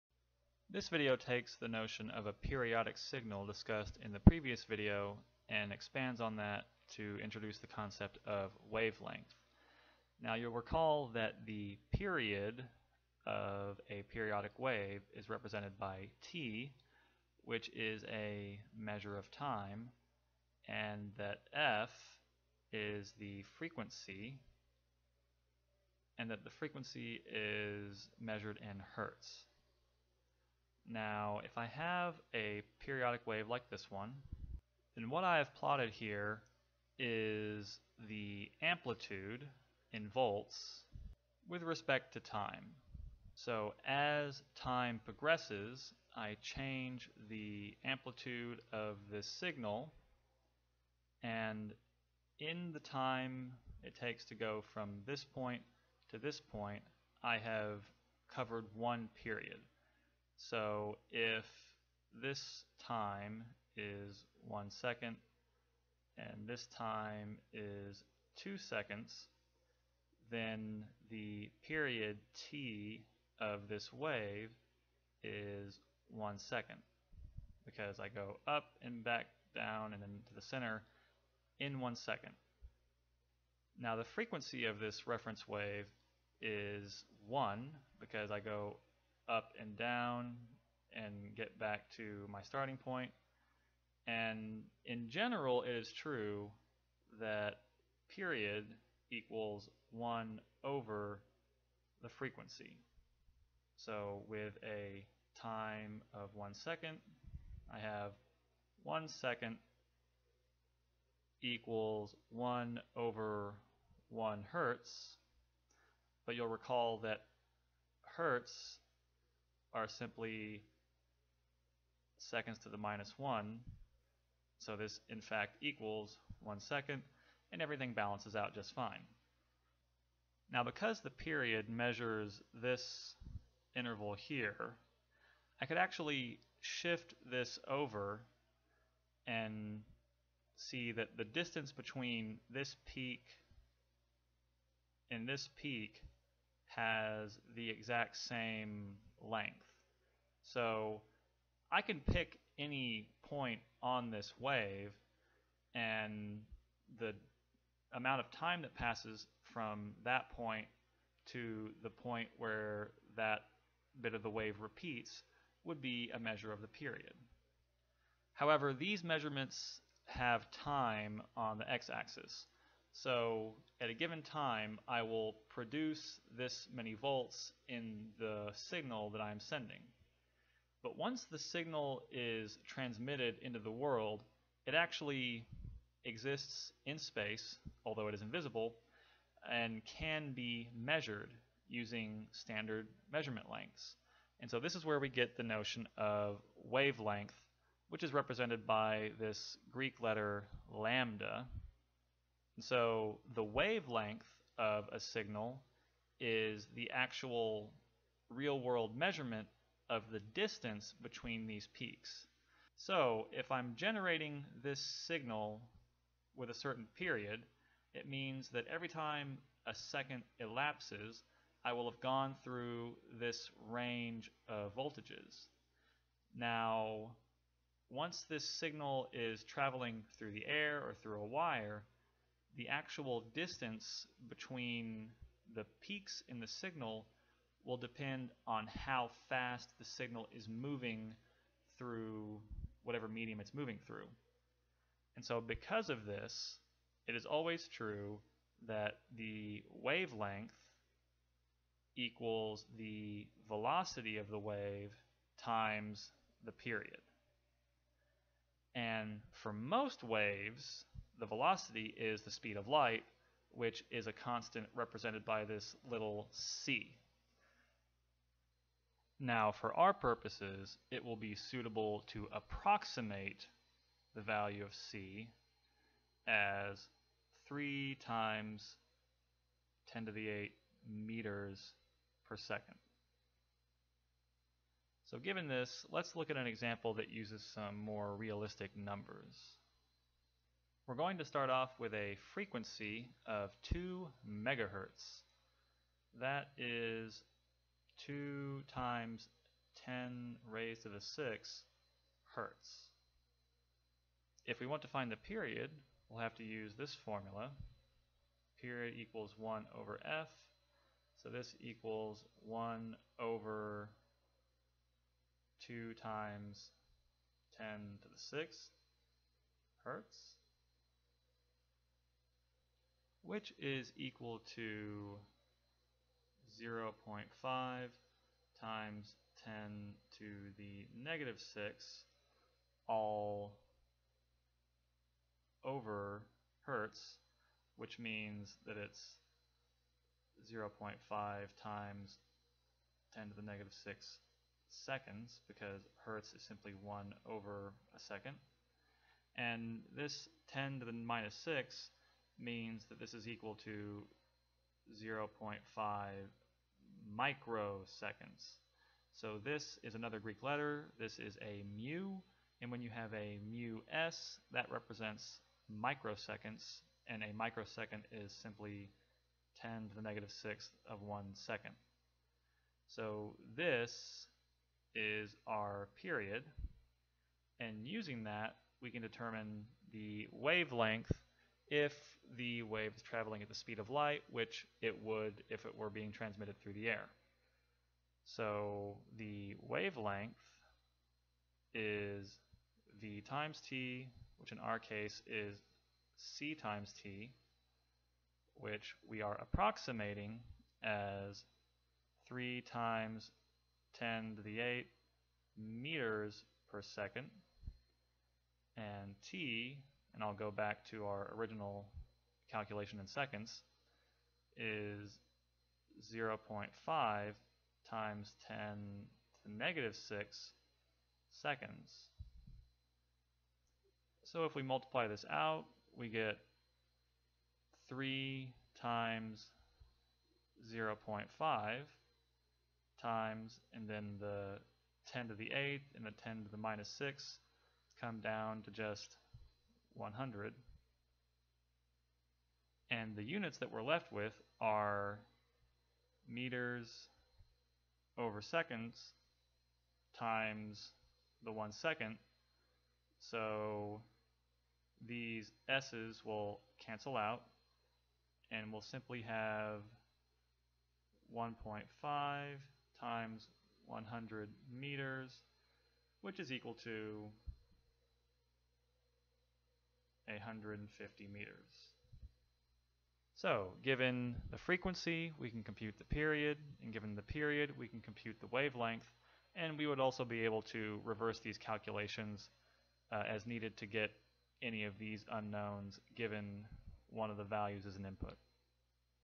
Data Transmission 2: Wavelength
The concept of wavelength is introduced and related to period and frequency. Much of this material is based on content from the book Data and Computer Communications by William Stallings.